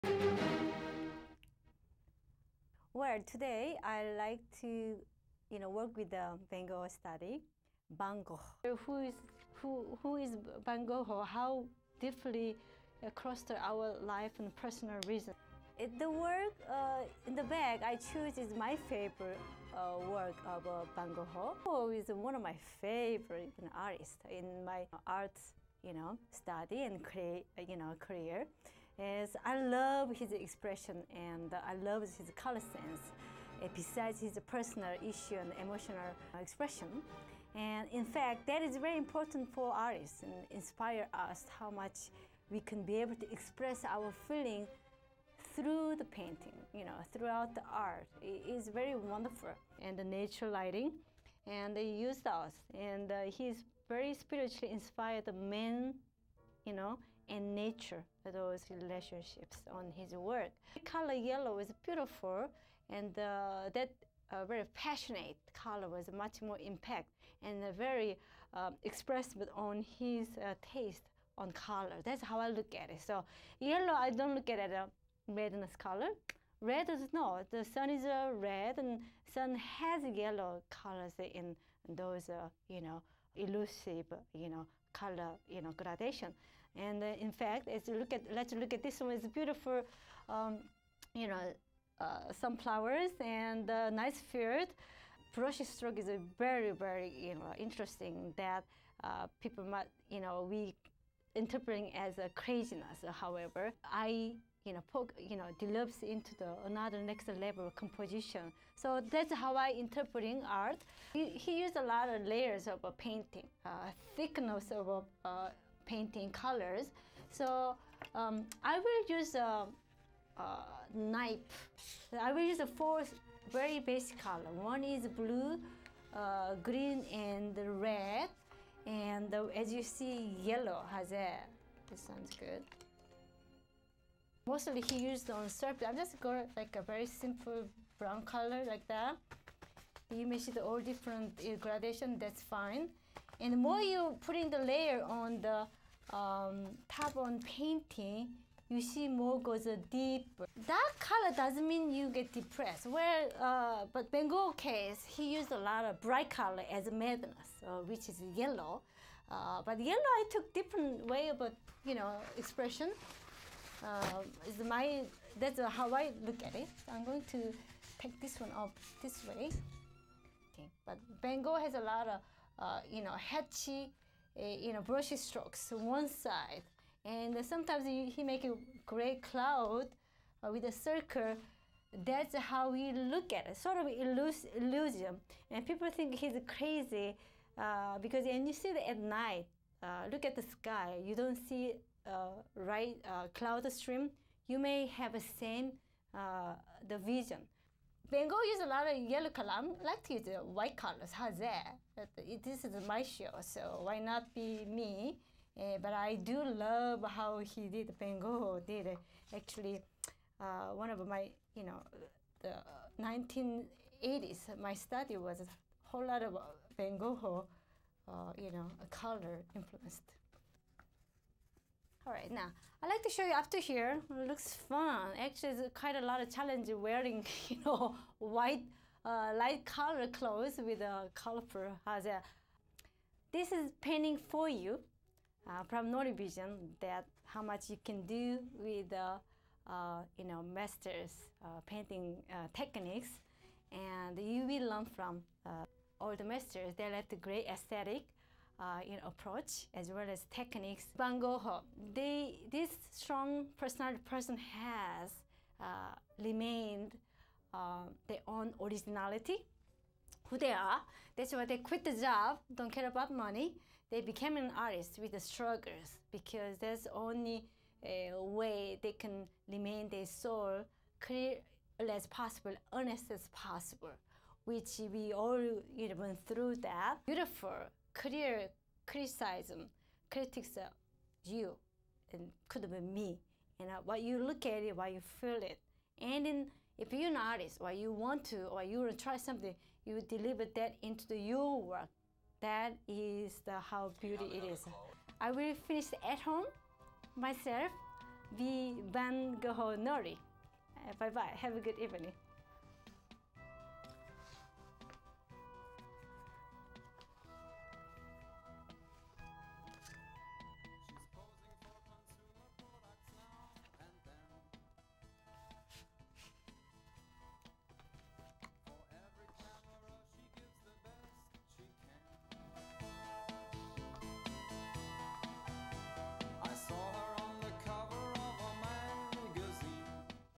Sketch with Van Gogh
All about Vincent Van Gogh and encouraging NoriVision viewers to see current Gogh show at MoMA, NYC in Fall.
Inspired by Van Gogh paintings and study into new mordern way of painting creation.

History mentioned of Van Gogh's background, techniques, personal story and details of his personal life as an artist.

Any contribution accepted improving, Live art TV show: P.O.Box#223, NYC 10013